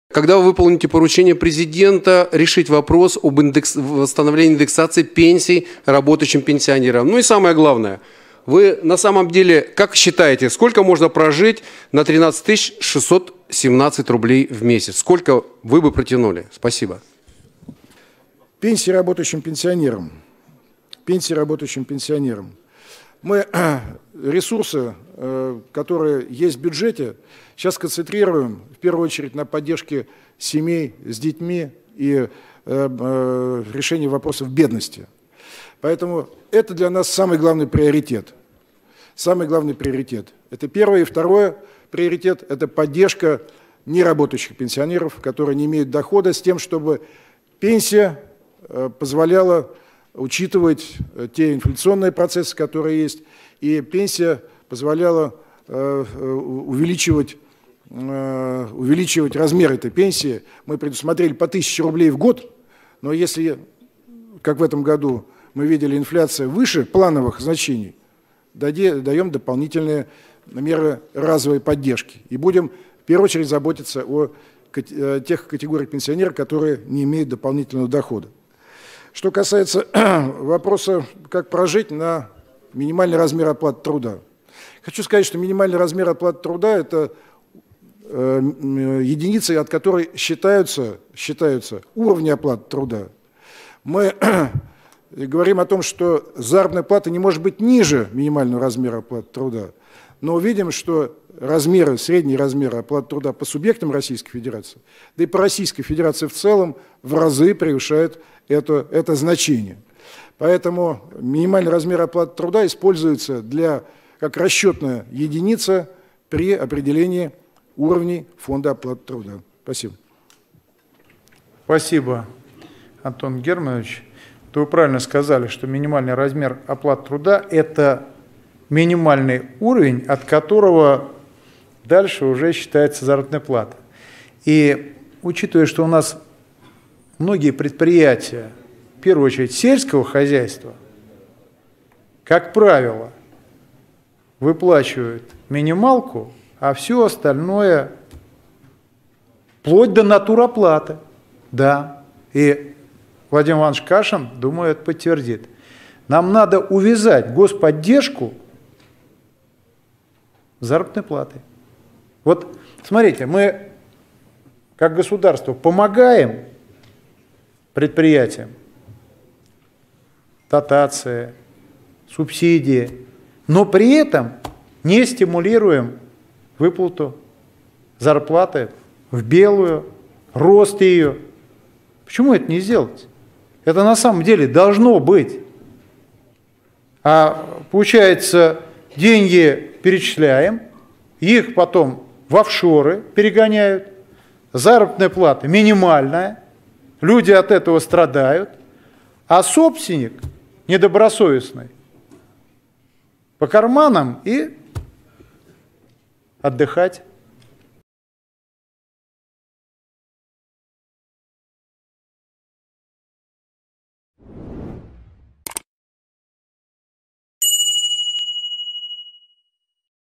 Валерий Гартунг "пригвоздил" вопросами министра. На помощь Силуанову пришел Володин.
Когда будет выполнена индексация работающим пенсионерам? Это поручение президента! Сколько Вы бы "протянули" на 13 617 рублей?

Друзья, буду благодарен Вам за поддержку моей работы - Raiffeisen BANK  - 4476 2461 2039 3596